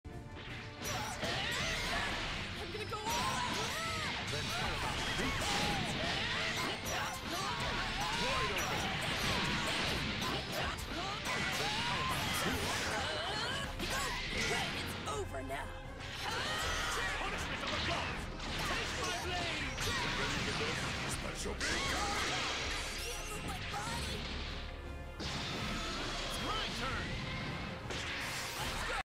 S2 1.19 TGohan + Piccolo + Black 5[L]LL OOC ToD
For Globo with the help of Houdeani 8/11

0 bar start (Sparking Lv1, [ ] = Whiff)
5[L]LL Delay Spark 5[L]LL JM(Black Assist)H 1S 5H JLMH J236L Hold (Piccolo Assist*) 1S 2S JLMH J236L Hold (Black Assist*) DR Hold 214M AA Kamehameha Black Lv1 Special Beam Cannon

* Call Piccolo Assist a bit before landing
* Call 2nd Black Assist a few hits into J236L

For Corner, don't delay the spark.